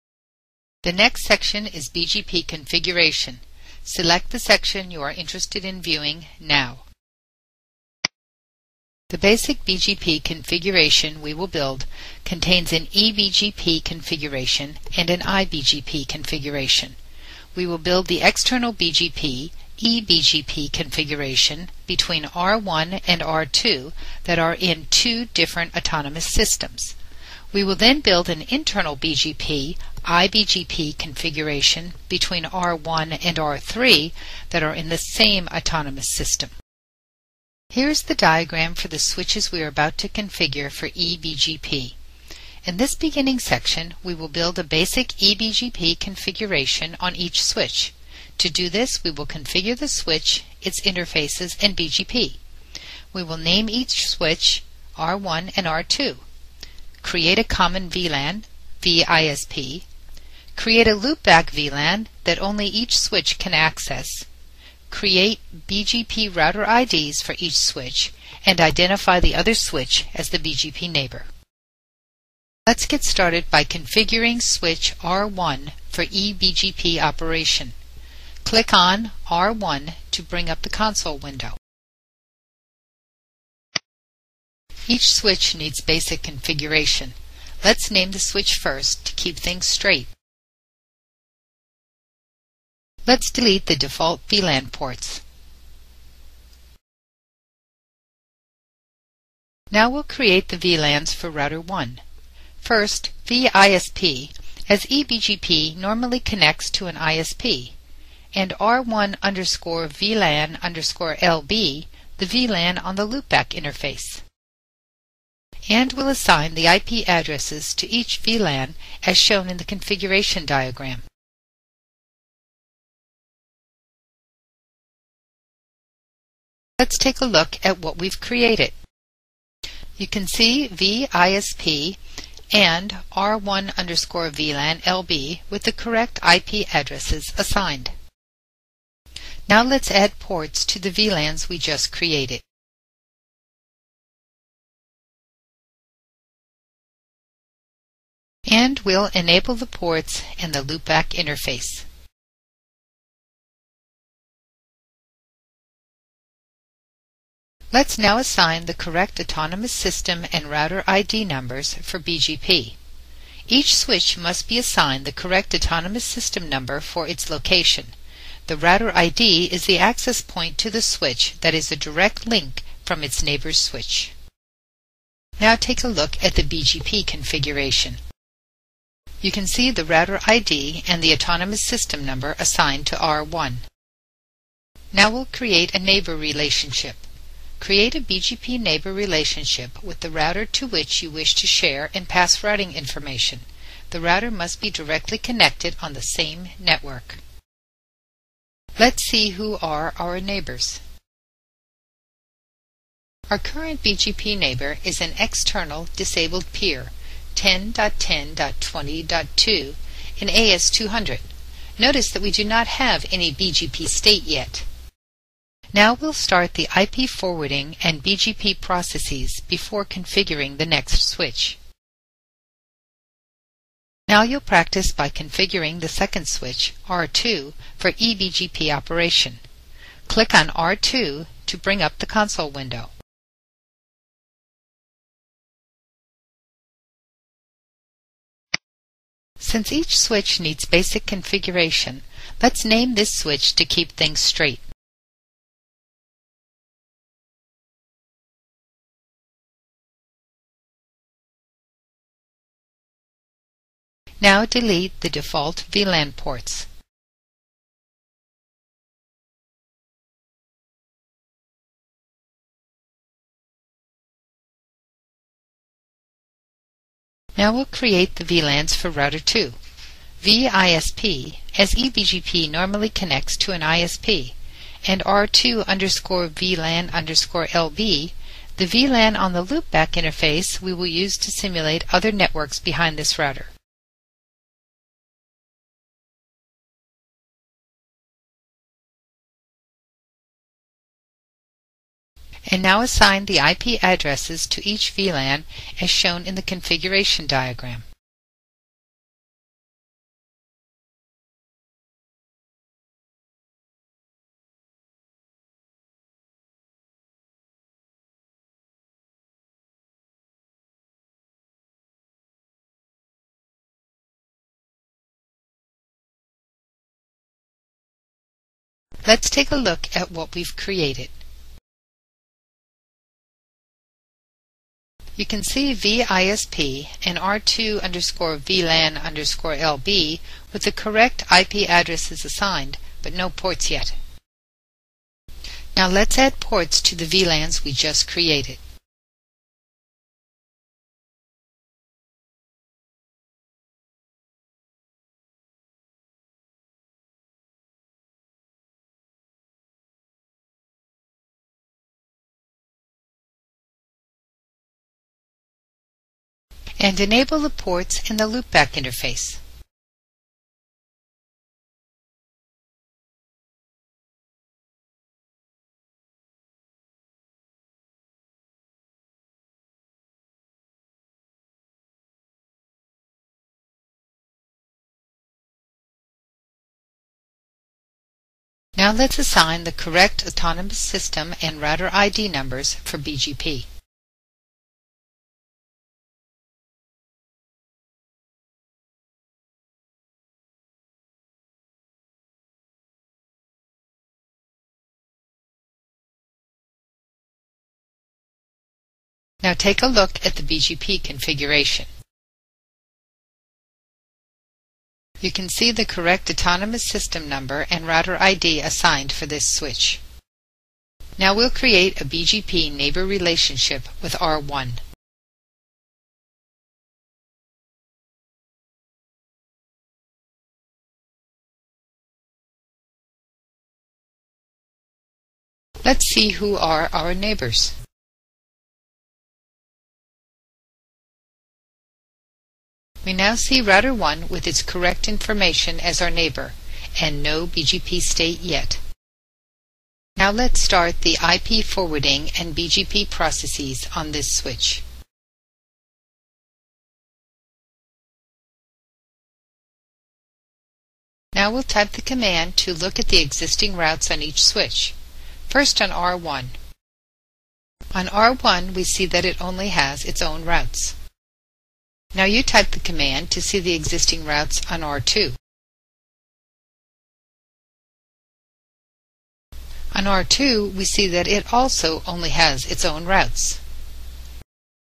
ExtemeXOS-Border Gateway Protocol BGP Basics - Part 3
Part 3 - This video is a capture of the interactive online training module, ExtremeXOS Border Gateway Protocol (BGP) Basics